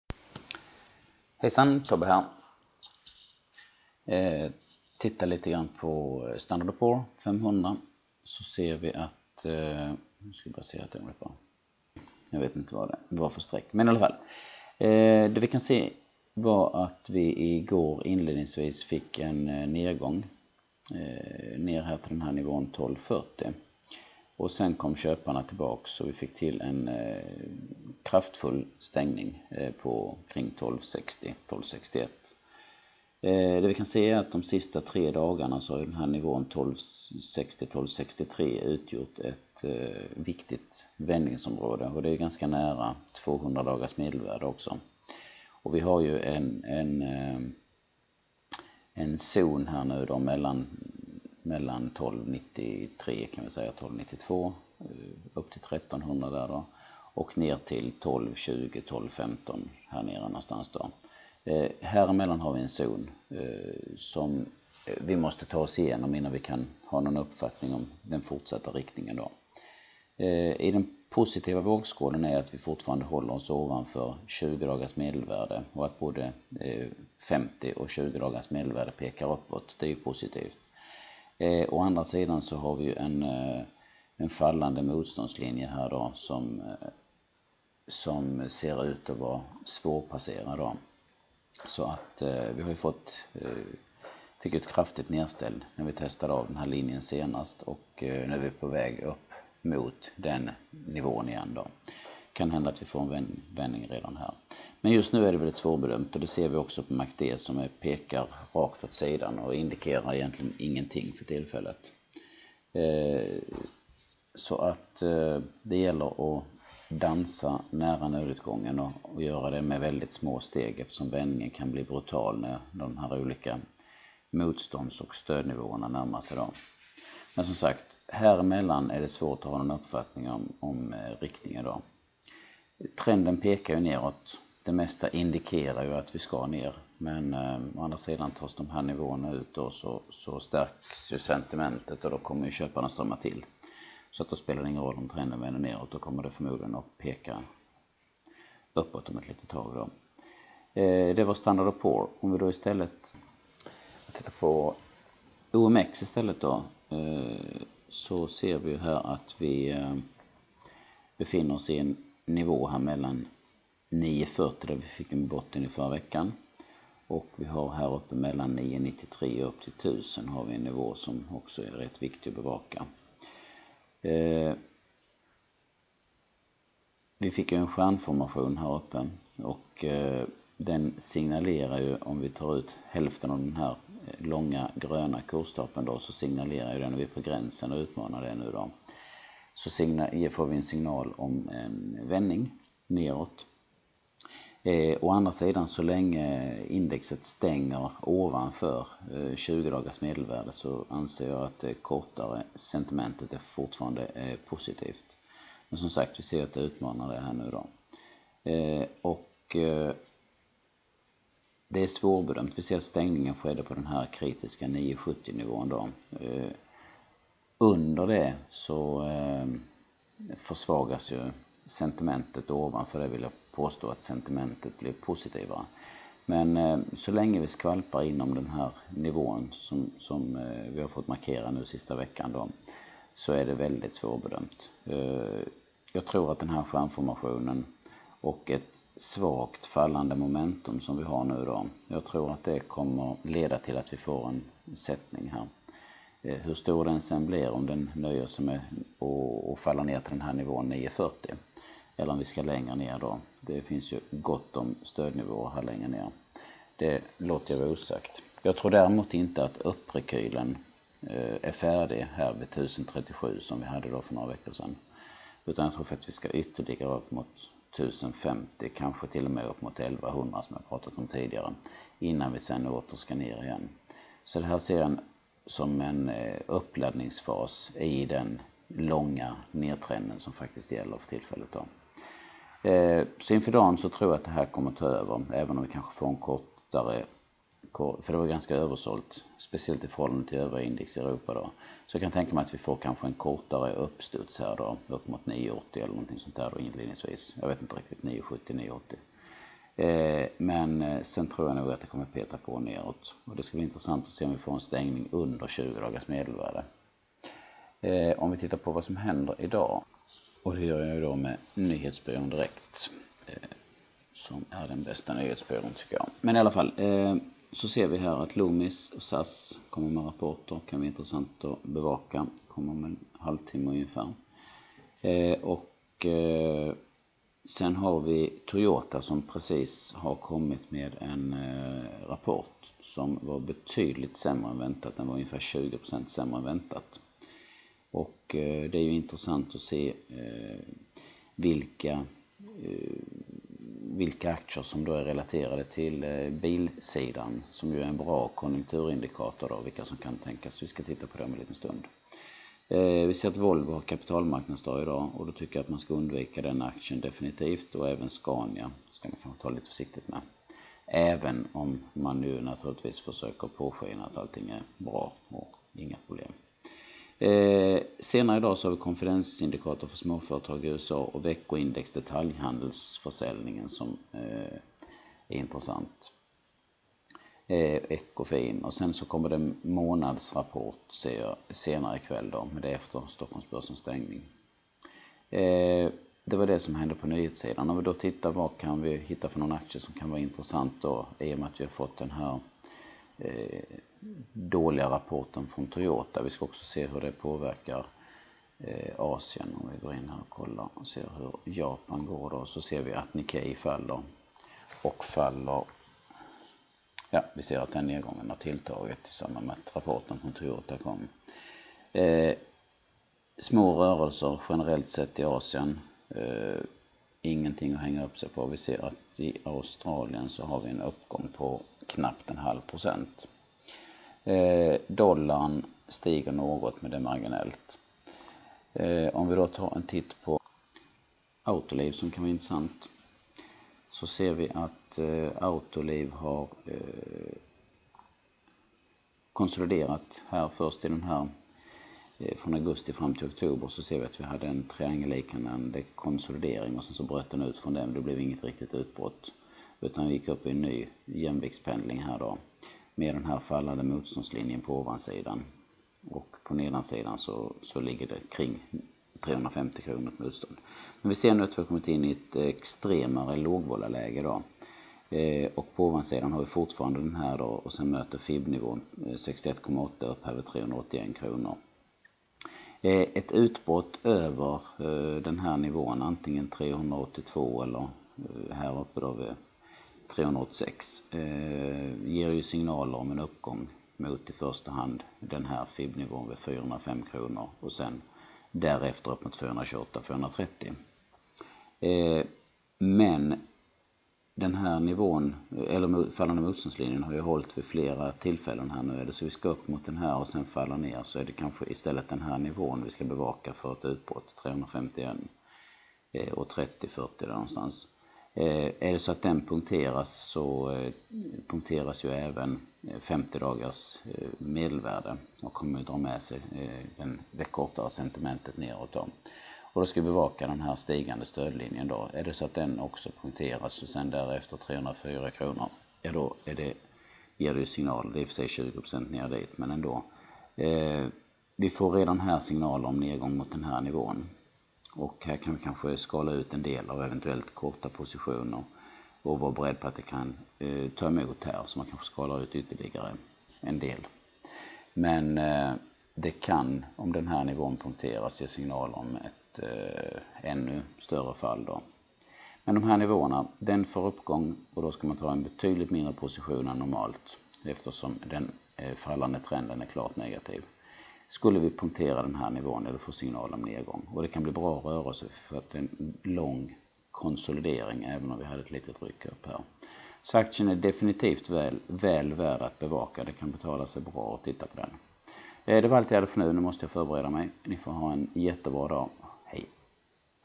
111108 OMXS30 SP500 Autoliv.avi
Den svaga utvecklingen på den svenska börsen igår kan utan vidare spegla inledningen av tisdagen. S&P 500 kämpar med att ta sig upp igenom motståndsområdet mellan 1293 och 1220. Situationen är likartad på Stockholmsbörsen som behöver ta ut i första hand motståndet 990 eller stödet 940 för att ge en hint om fortsättningen. Toyota redovisade en betydligt sämre vinst än väntat och det kan utan tvekan påverka aktier som är relaterade till fordonsindustrin.